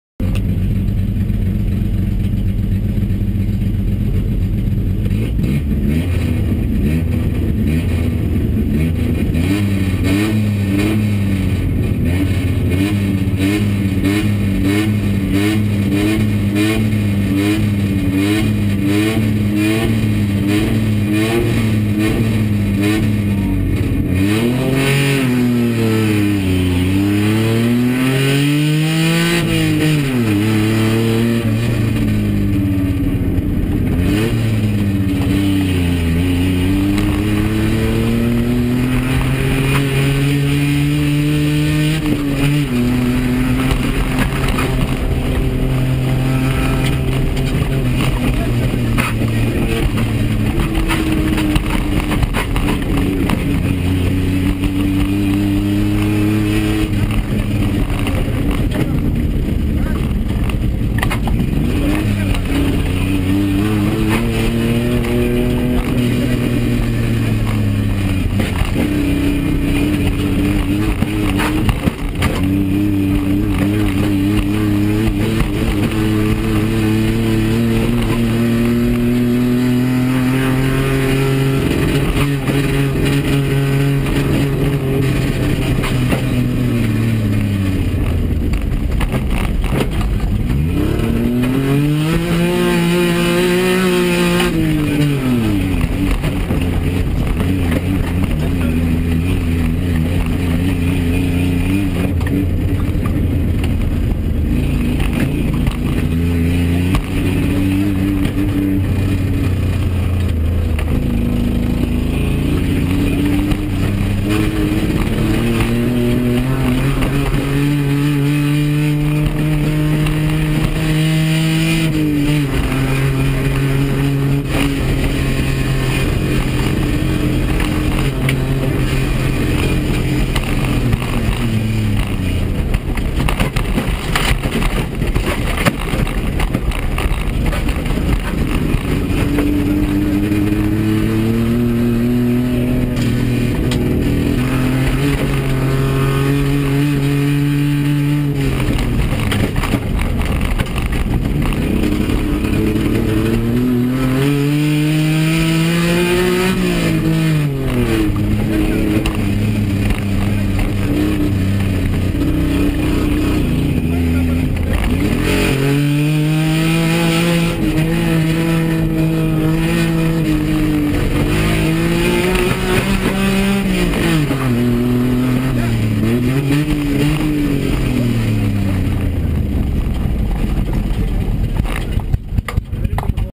5&5 Prime de calificacion rallycross 2011
El 5&5 de Sandro y Santiago Alban en el super prime de calificacion en el rallycross de Latacunga, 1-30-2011